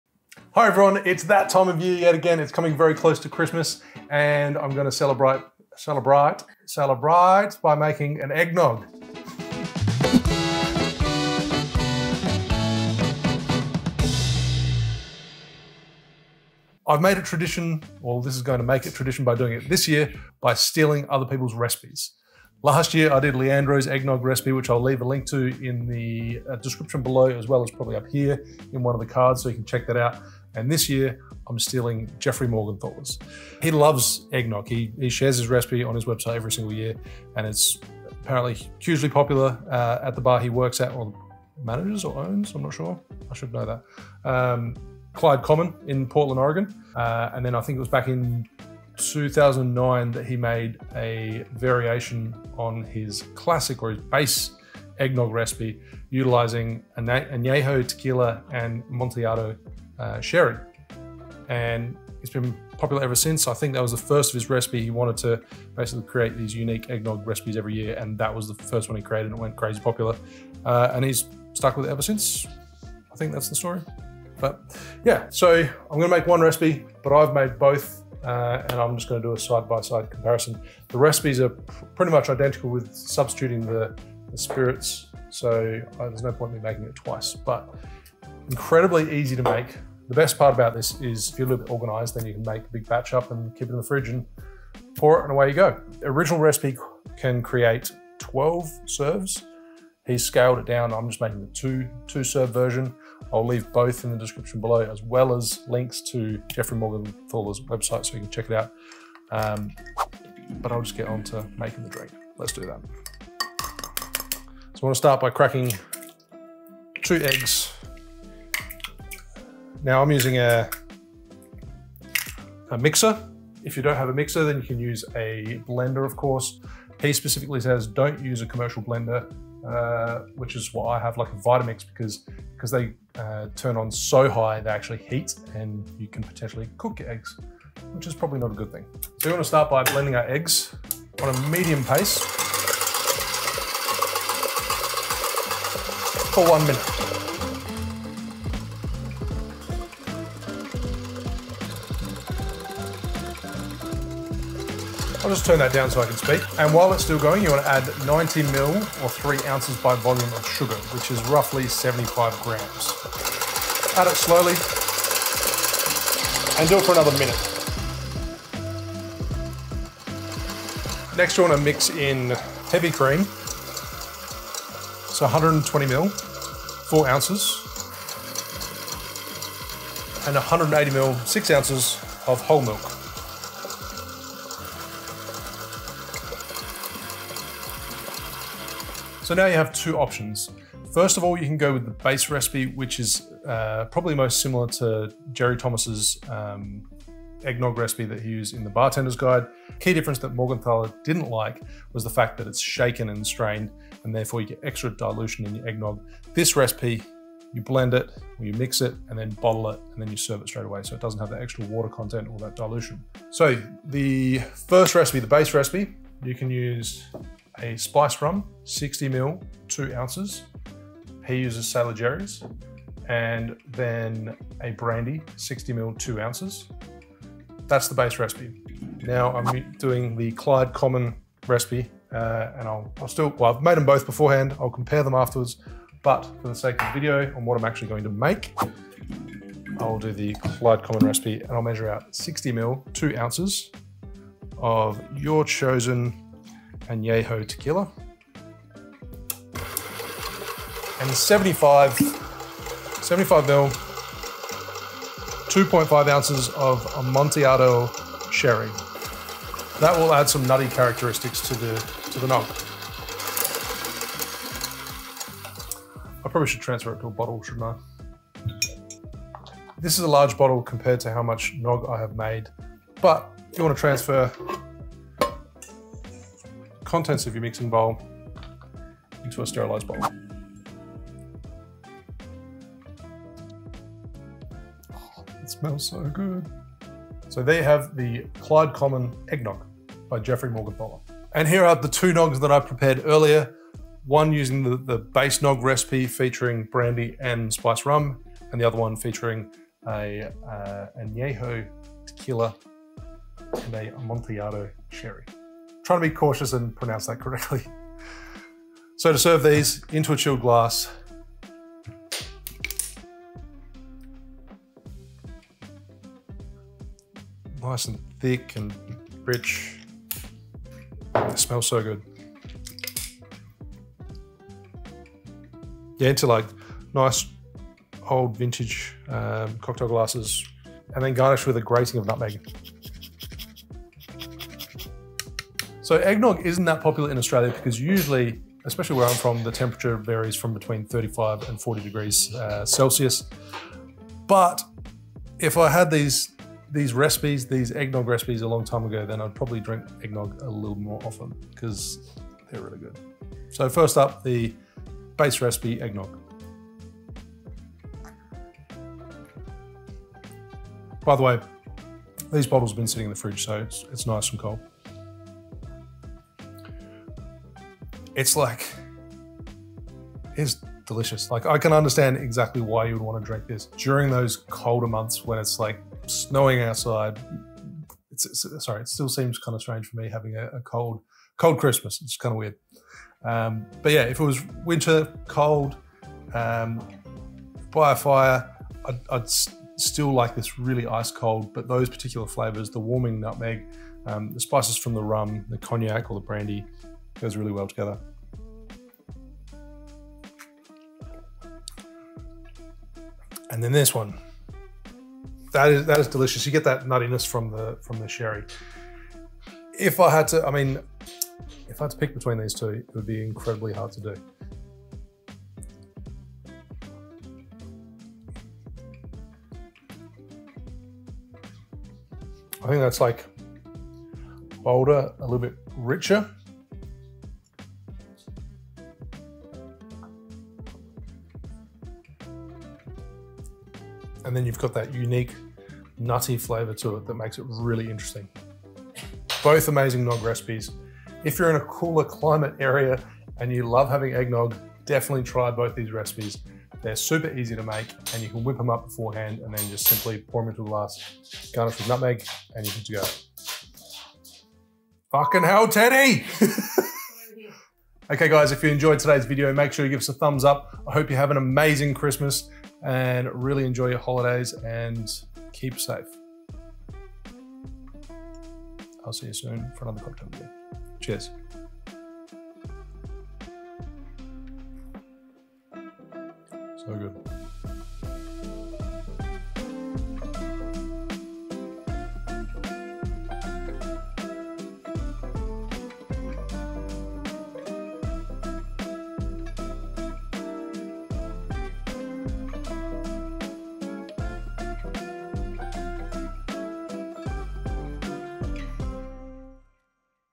Is this the greatest EGGNOG recipe of all time?
Time to steal another NOG recipe! This year I'm hijacking not one but two of Jeffrey Morgenthaler's eggnog recipes.. first of which is a base nog recipe and second is a unique tequila & sherry eggnog resulting in a nutty twist.

Want to check out last year's nog that I stole from Leandro (The Educated Barfly)?

The recipes listed below are the scaled recipes for 2-3 serves but you can find the larger scaled recipes

EGGNOG (BASE RECIPE)
- 2 large eggs
- 90ml (by volume) superfine or baker's sugar (3 oz)
- 60ml Brandy (2 oz)
- 60ml Spiced Rum, Sailor Jerry’s (2 oz)
- 180ml Whole Milk (6 oz)
- 120ml Heavy Cream (4 oz)

CLYDE COMMON EGGNOG
- 2 large eggs
- 90ml (by volume) superfine or baker's sugar (3 oz)
- 60ml Añejo Tequila (2 oz)
- 75ml Amontillado Sherry (2.5 oz)
- 180ml Whole Milk (6 oz)
- 120ml Heavy Cream (4 oz)

🍹 RECOMMENDED BARWARE:
- Cocktail Shaker
- Hawthorn Strainer
- Japanese Jigger
- Citrus Press
- Mixing Glass
- Mallet / Lewis Bag Set
- Clear Ice Mould